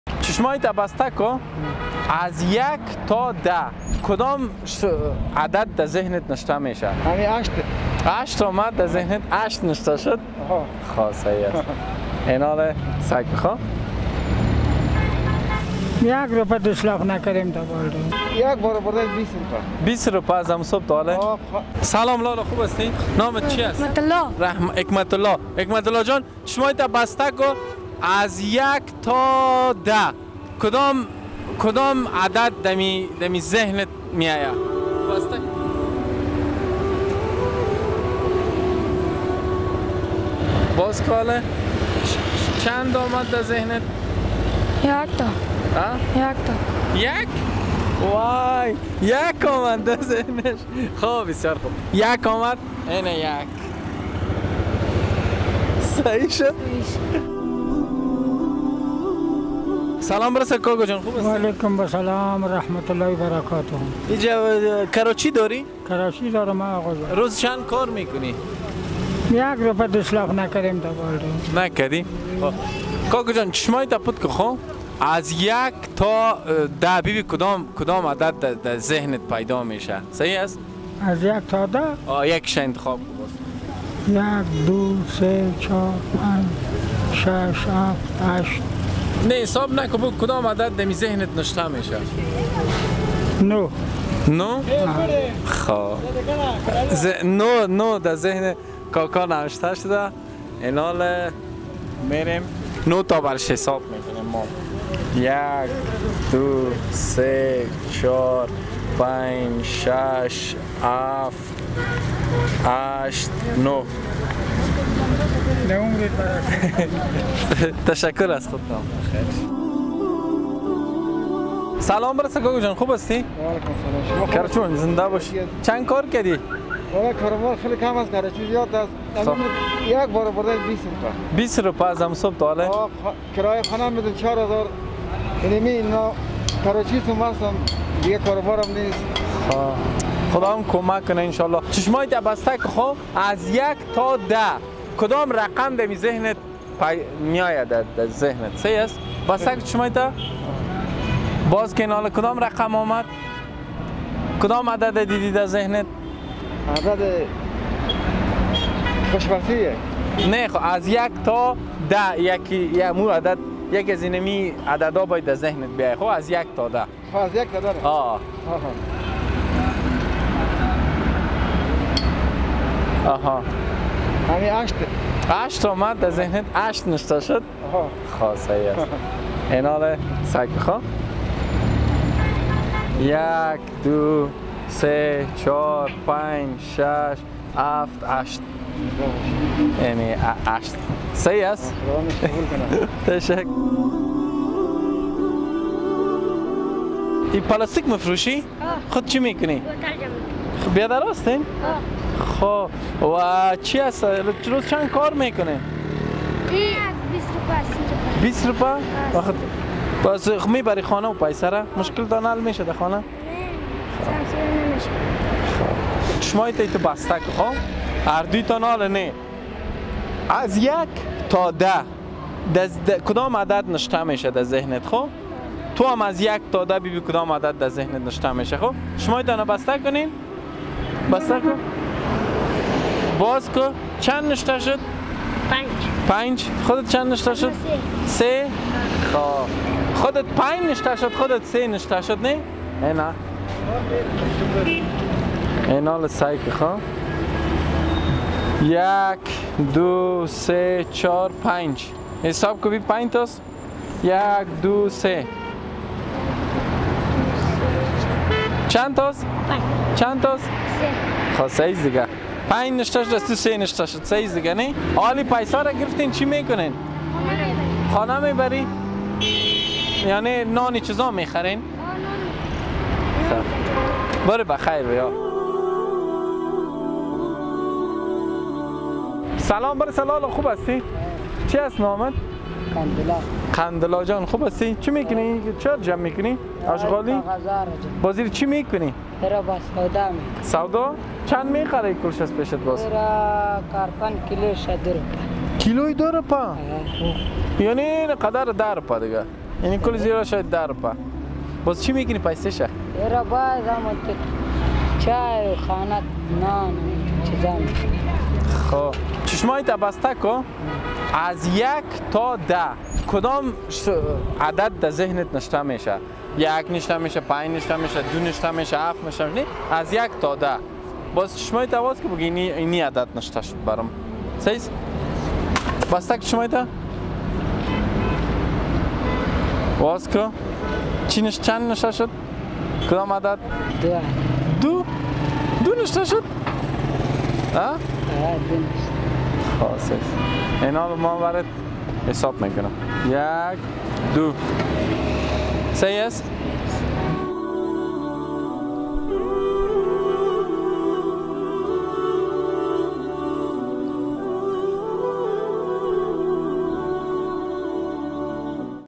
چشم هایت بسته کو کدام عدد در ذهن ات آمد؟!
چشم هایت بسته کو
کدام عدد در ذهن ات آمد؟!
برنامه تفریحی بابت دستگیری و دل جویی از برادران نیازمند ما
خوشا آنانیکه برای آوردن لبخند به لب همنوعان شان بهانه می جویند.